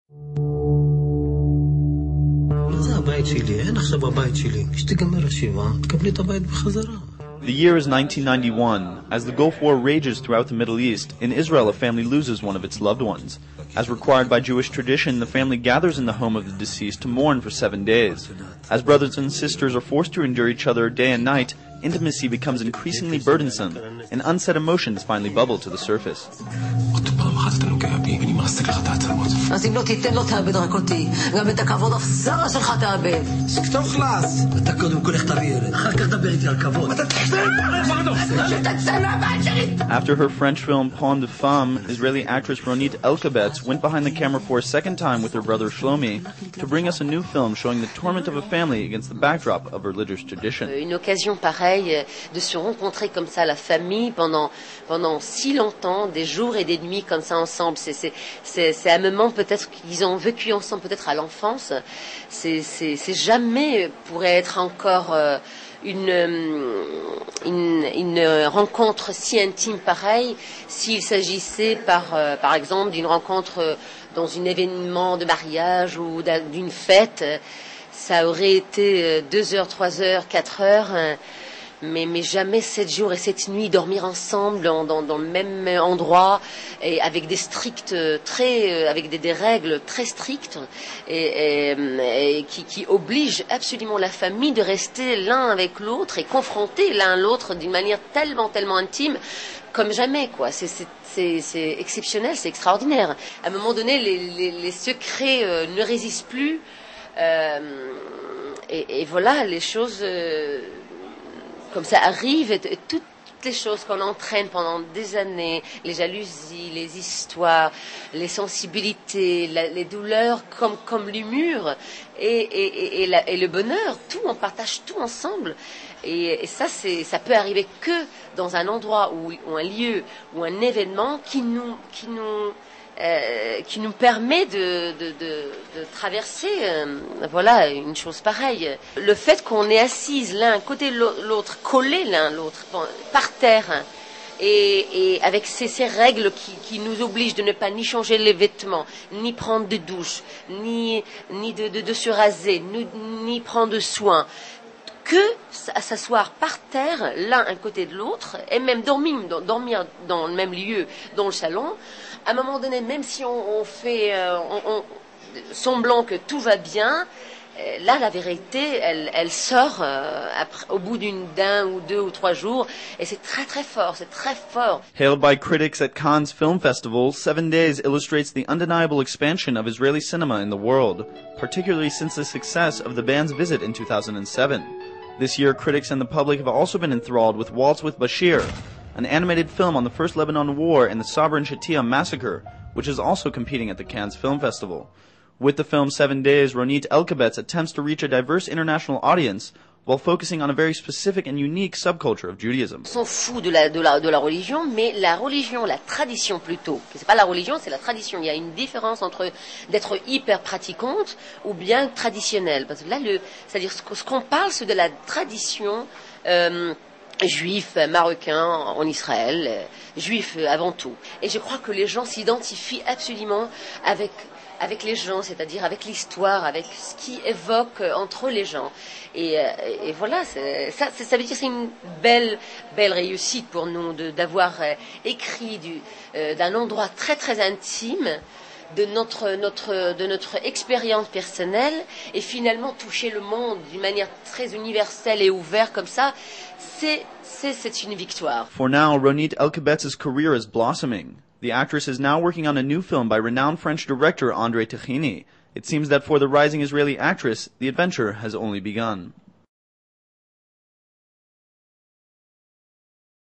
Infolive.tv Exclusive Interview With Israeli Actress Ronit E
Infolive.tv sat down with rising Israeli star Ronit Elkabetz to discuss her blossoming career, her new film &#39;7 Days&#39; and her plans for the future. 07/21/08